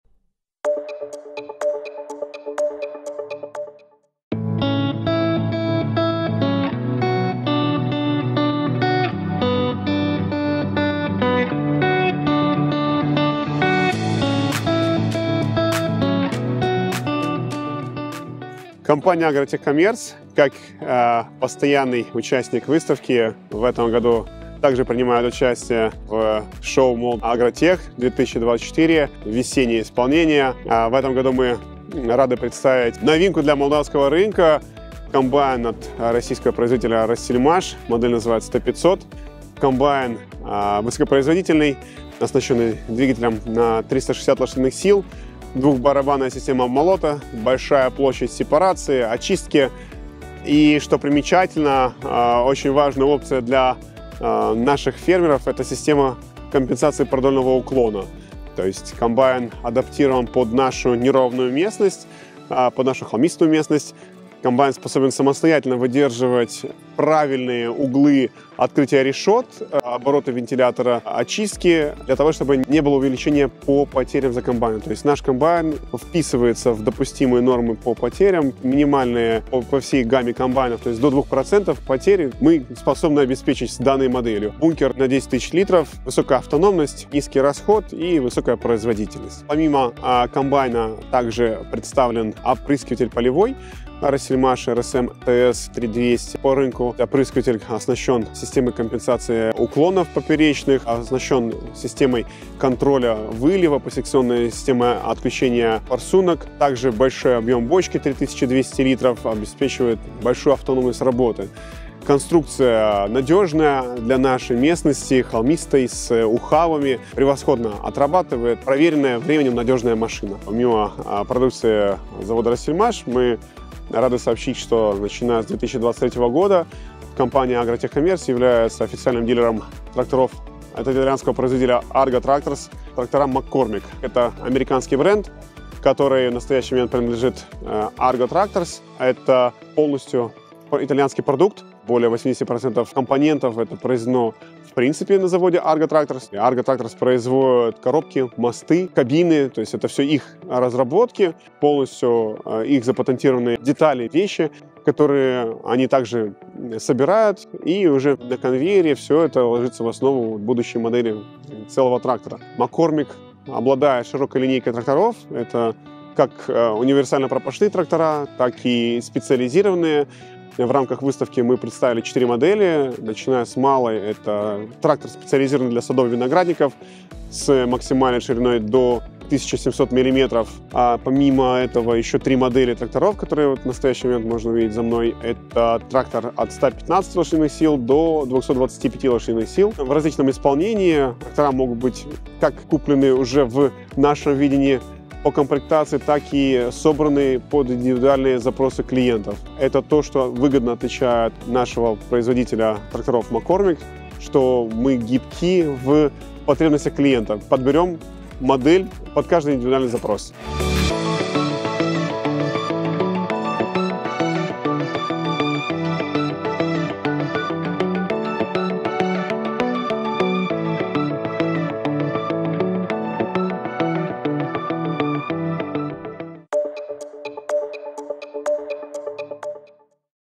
Новинки сельхозтехники от ATC-Agrotehcomerț на «Молдагротех-2024»
Компания ATC-Agrotehcomerț, на проходящей в МВЦ "Молдэкспо" выставке "Молдагротех-2024", по традиции представила широкую гамму сельхозтехники от производителей разных стран, в том числе ряд новинок. / ATC-Agrotehcomerț, în cadrul expoziției Moldagrotech-2024, desfășurată la Moldexpo IEC, a prezentat în mod tradițional o gamă largă de mașini agricole de la producători din diferite țări, inclusiv o serie de produse noi.